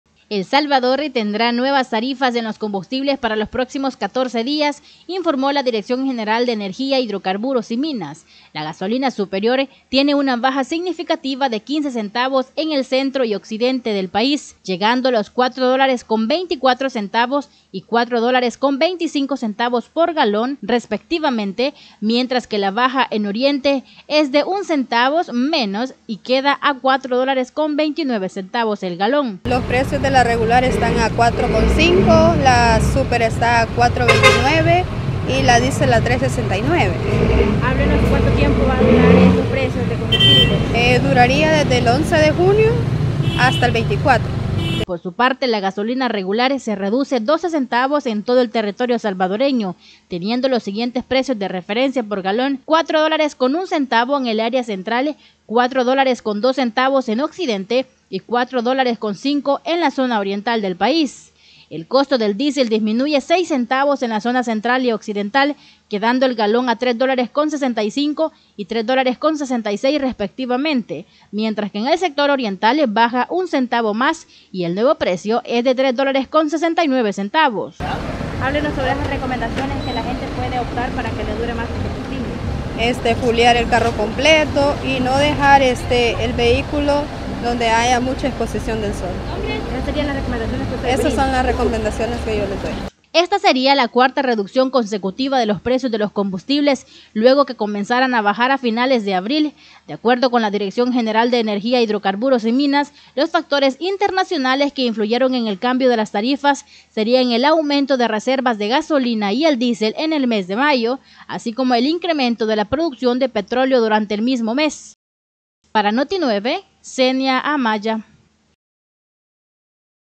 NUEVOS PRECIOS EN LOS COMBUSTIBLES
La Dirección de Energía, Hidrocarburos y Minas, informó sobre los nuevos precios de referencia del combustible, reflejando por tercera ocasión una disminución en los precios que estarán vigentes hasta el 24 de junio.